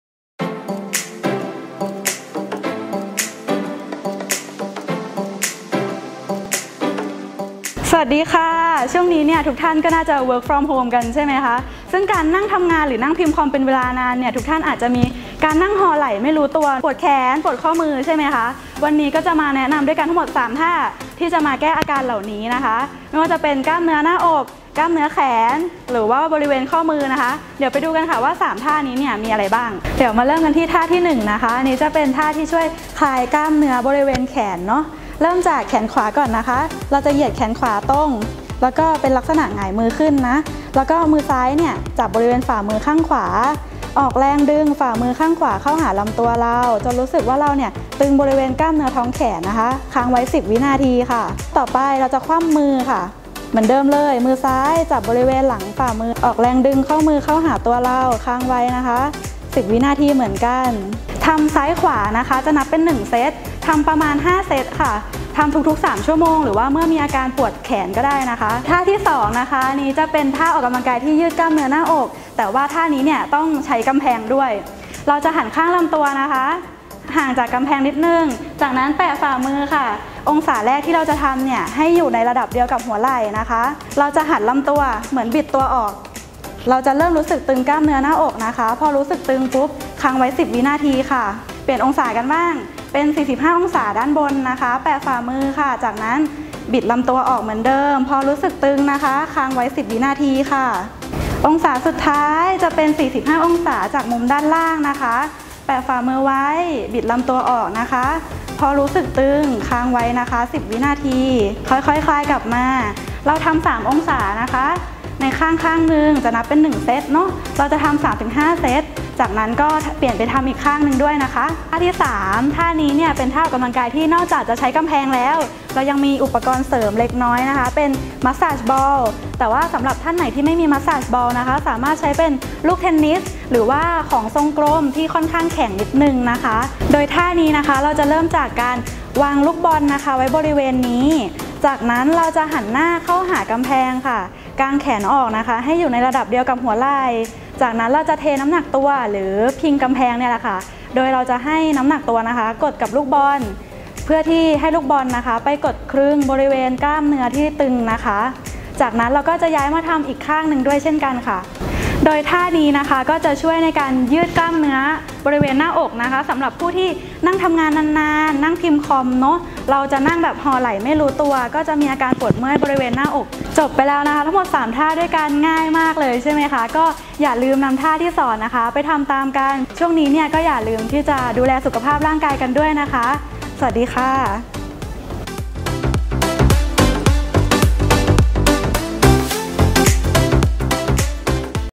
Home exercise (for Chest,Wrists and Forearms) by Pain Away | Ep.5 Physiotherapy Bangkok
These days everyone must be working from home,
Which sitting or using computer for a long time could cause
a rounded shoulder posture without noticing or pain in your arms and wrists.

So, I am going to show you 3 exercises
that can relieve pain from Chest(Pectoralis muscles), Biceps and Forearms( Brachioradialis muscles).
Let’s see the exercises!

By Panida Tangpiyanan
ATTM.D.
ทุกวันนี้นะคะจากการที่หลายๆคนนั่งทำงานจากที่บ้านโดยเฉพาะ
การนั่งทำงานหน้าคอมพิวเตอร์เป็นเวลานาน อาจส่งผลให้เกิด
การนั่งห่อไหล่โดยไม่ทันรู้ตัว และอาจส่งผลให้เกิดปัญหาอาการปวด
บริเวณไหล่ อก แขน และข้อมือได้อีกด้วย
วันนี้ โบว์เลยจะมาสอนท่าออกกำลังกายง่ายๆ 3 ท่าทำได้เองที่บ้าน
ซึ่งจะช่วยยืดกล้ามเนื้อบริเวณอก แขน และข้อมือ โดยอาศัยแค่ฝาผนังบ้าน
และลูกบอลเล็กๆอีกลูก ก็ทำได้แล้วละค่ะ
อย่ารู้แล้วใช่ไหมคะว่าทำอย่างไร??? ไปดูกันเลย!!

พท.ป.พนิดา ตั้งปิยะนันท์
แพทย์แผนไทยประยุกต์